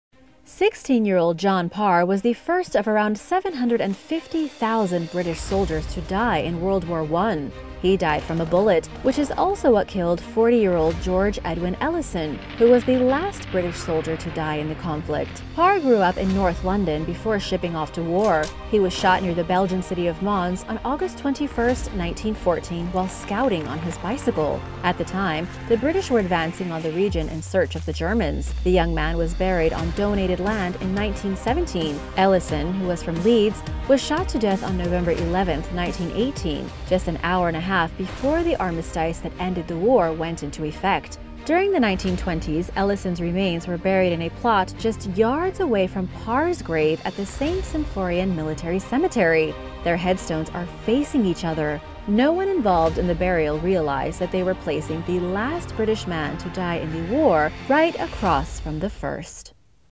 Historical Coincidence Soldiers WWI
Sixteen-year-old John Parr was the first of around 750,000 British soldiers to die in World War I. He died from a bullet, which is also what killed 40-year-old George Edwin Ellison, who was the last British soldier to die in the conflict. During the 1920s, Ellison’s remains were buried in a plot just yards away from Parr’s grave at the St. Symphorien Military Cemetery. Their headstones are facing each other. No one involved in the burial realized that they were placing the last British man to die in the war right across from the first.